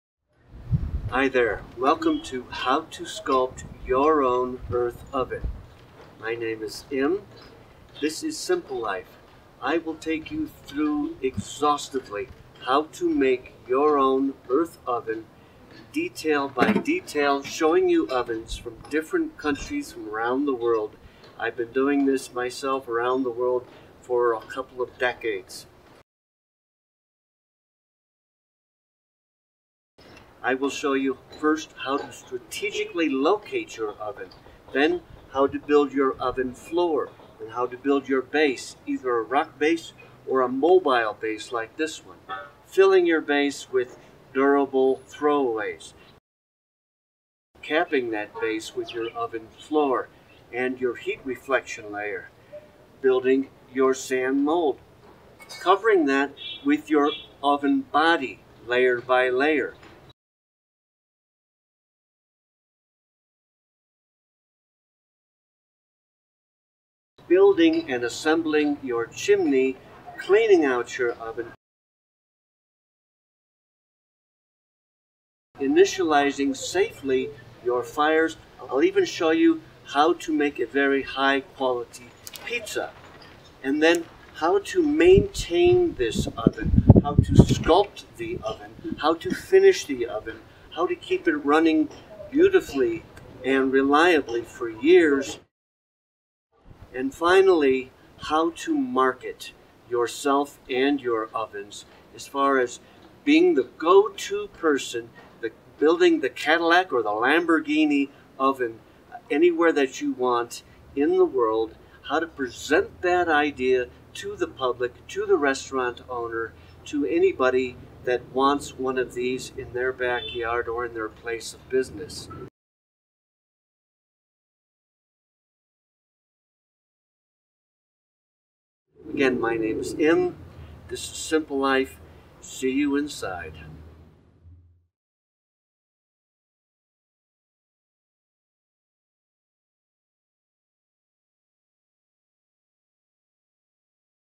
How To Sculpt Your Own Earth Oven Intro- SimPLife
spanning 4 countries over 16 years, this creative design and business opportunity, taking over 3 years to create, will show you in succinct detail, how to design, build and offer Earth Ovens to the world, and to be the defacto premium oven-builder in your area. from here, its all up to you. SimPLife intends for this not only to improve the quality of your food experience, but to offer you new directions in creative expression, new ways to engage business, and to establish new business and guild possibilities.